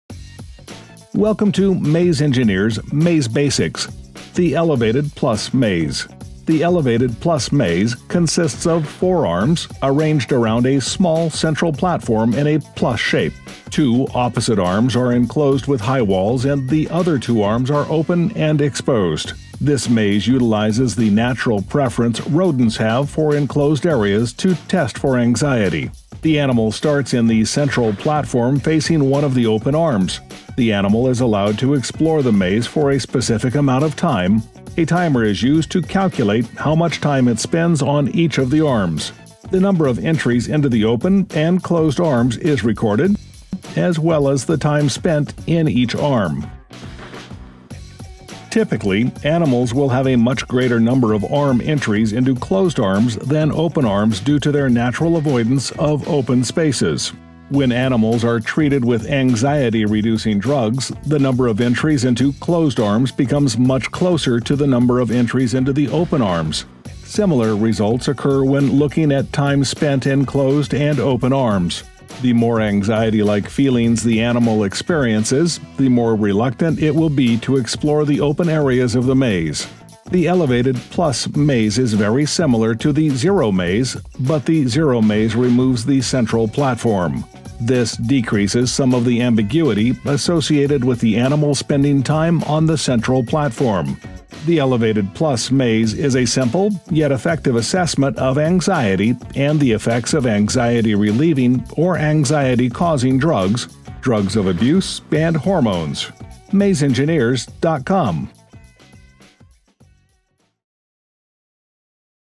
Elevated Plus Maze
The Elevated Plus Maze is a widely used assay for anxious behavior in rodents. The assay leverages rodent’s natural behaviors to provide a useful experimental analog for anxiety.

For more information on Elevated Plus Maze Laboratory Apparatus, check out our portfolio